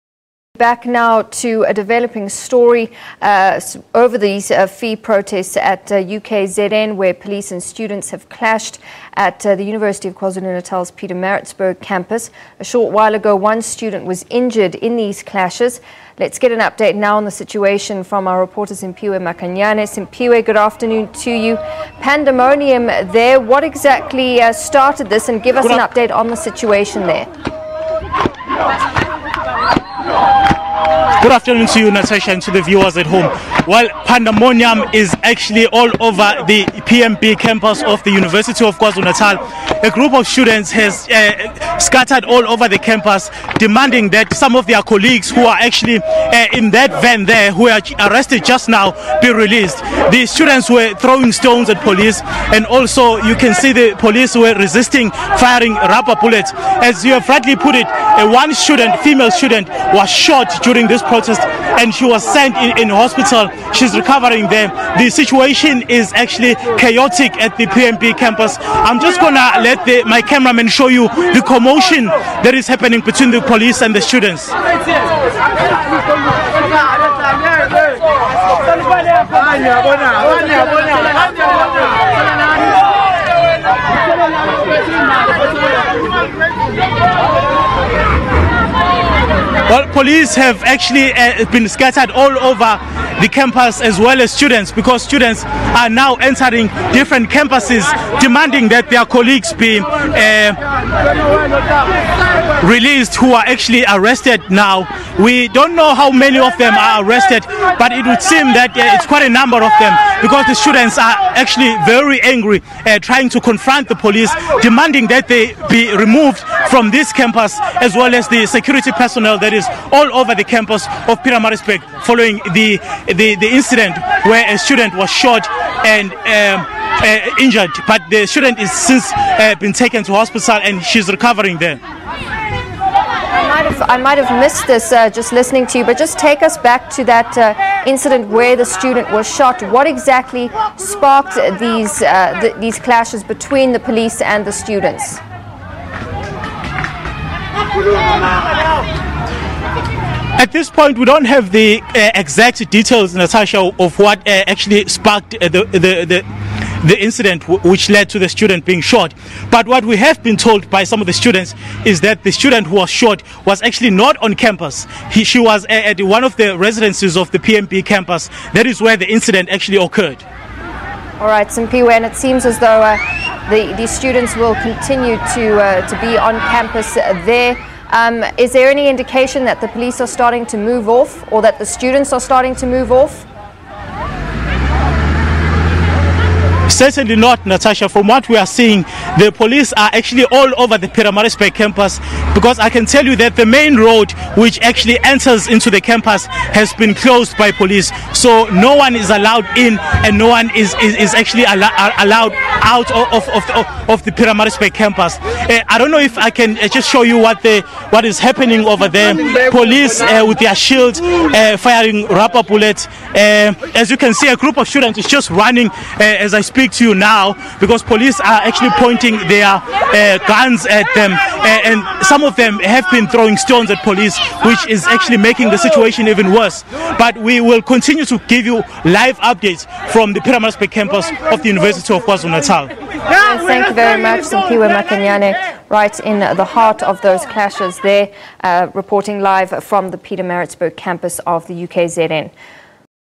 Police and students clash at the University of KwaZulu-Natal
Police and students have clashed at the University of KwaZulu-Natal's Pietrmaritzburg campus.
A short while ago, one student was injured in the clashes.
We get the latest on the situation there from our reporter Simphiwe Makhanya in Pietermaritzburg.